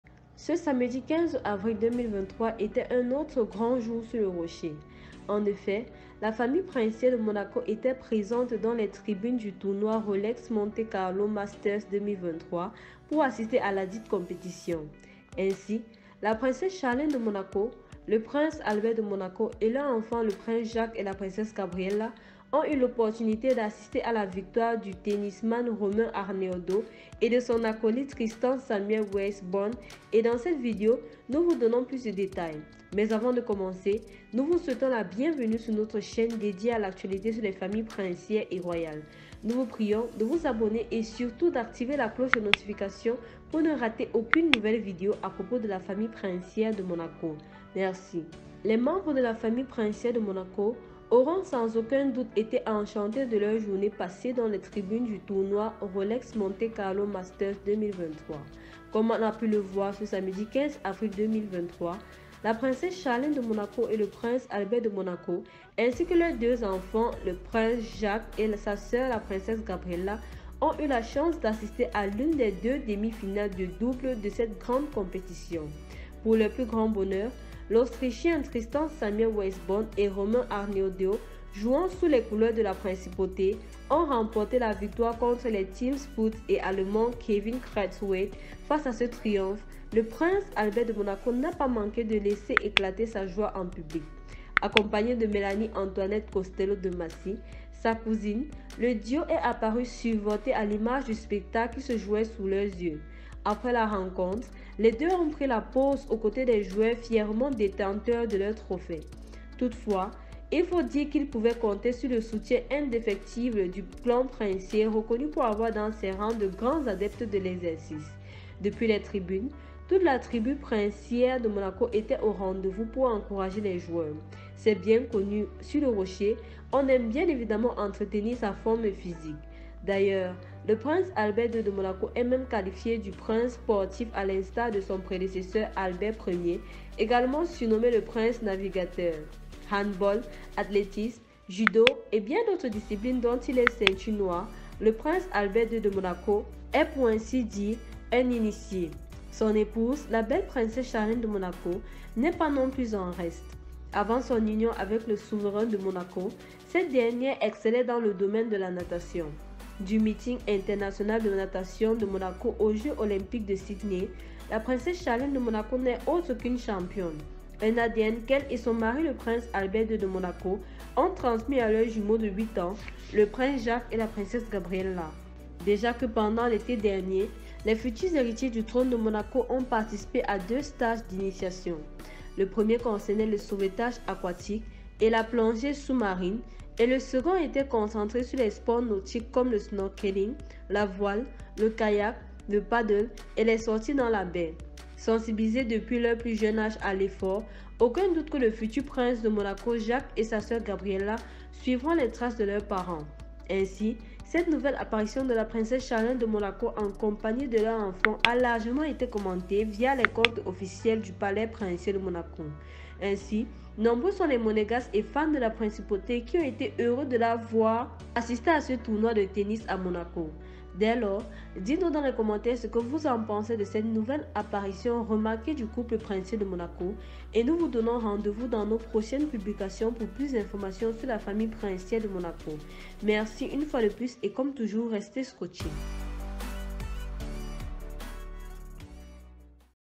Charlene de Monaco et Albert assistent à un tournoi de tennis avec Jacques et Gabriella
Charlene de Monaco et Albert assistent à un tournoi de tennis avec Jacques et Gabriella. Ce samedi 15 avril 2023 était un autre grand jour sur le Rocher.  Ainsi, la princesse Charlene de Monaco, le prince Albert de Monaco et leurs enfants le prince Jacques et la princesse Gabriella ont eu l’opportunité d’assister au tournoi Rolex Monte-Carlo Masters 2023.

Comme on a pu le voir ce samedi 15 avril 2023, la princesse Charlene de Monaco et le prince Albert de Monaco ainsi que leurs deux enfants, la prince Jacques et sa sœur la princesse Gabriella, ont eu la chance d’assister à l’une des deux demi-finales de double de cette grande compétition

👇 Vidéo récentes sur Charlene de Monaco 👇

✅Charlene de Monaco heureuse et ravie par ce grand projet avec son mari Albert de Monaco